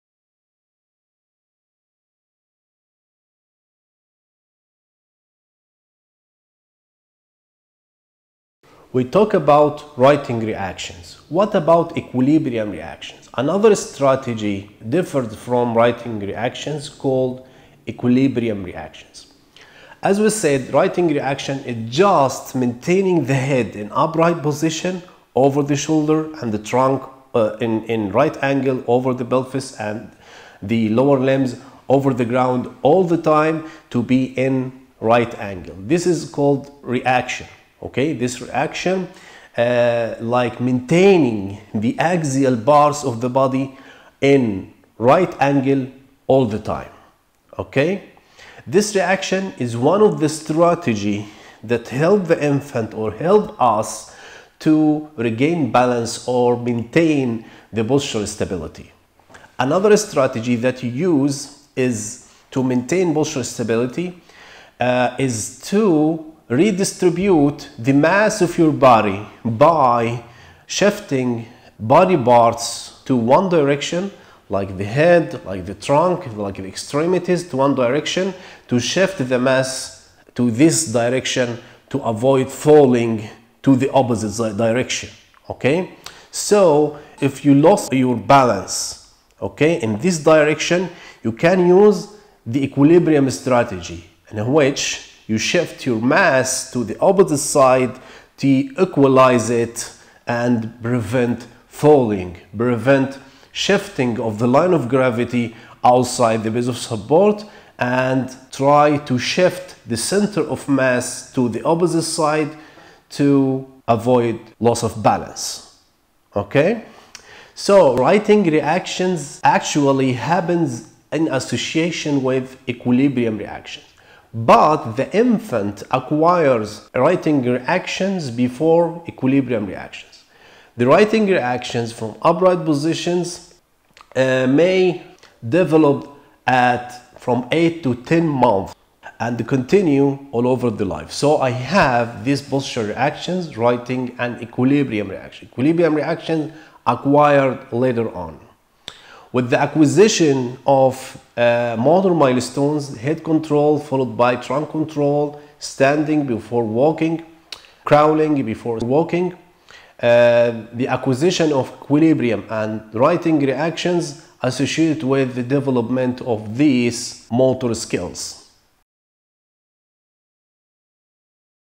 16 Difference between Righting & Equilibrium Reactions
In this video:
The differences between the two postural reactions (righting and equilibrium).